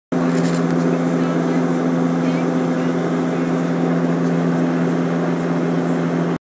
Flybe 1
Little Iphone clip on the inaugural flybe flight between manston and Edinburgh.
the 'effect' is due to the superior frame rate on the iphone lol